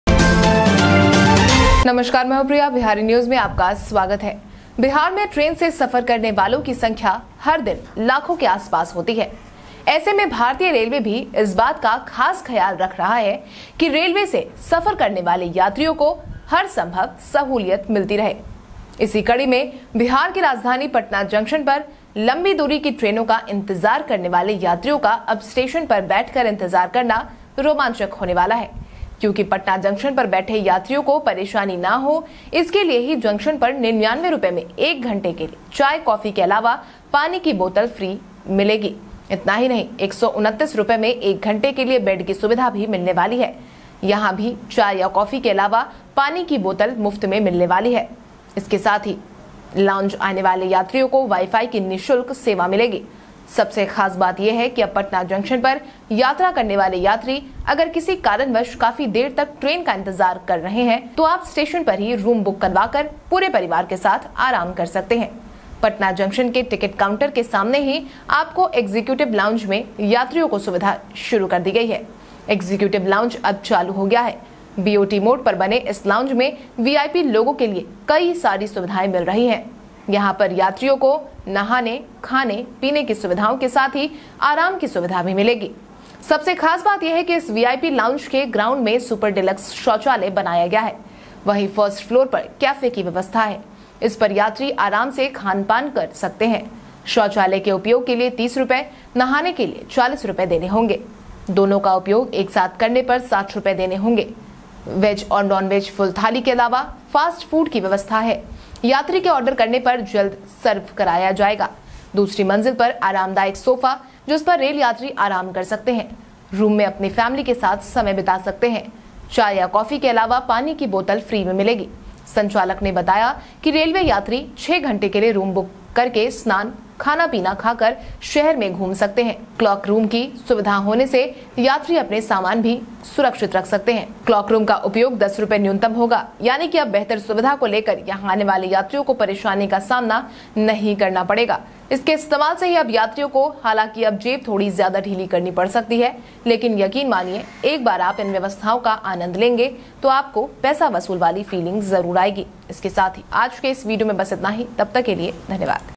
बिहार में यहाँ मिलेगा 99 रूपए में चाय कॉफ़ी के साथ साथ 1 घंटे वाई-फाई फ्री
The number of people traveling by train in Bihar is around lakhs every day, in such a situation, Indian Railways is also taking special care that passengers traveling by railways get all possible facilities. In this episode, it is going to be exciting for the passengers waiting for long distance trains at Patna Junction, the capital of Bihar, to sit at the station and wait for the passengers sitting at Patna Junction, so that the passengers sitting at Patna Junction should not be disturbed, only 99 rupees at the junction for one hour. In addition to tea and coffee, water bottle will be free, not only this, bed facility is also going to be available for 1 hour for Rs. Passengers will get free Wi-Fi service.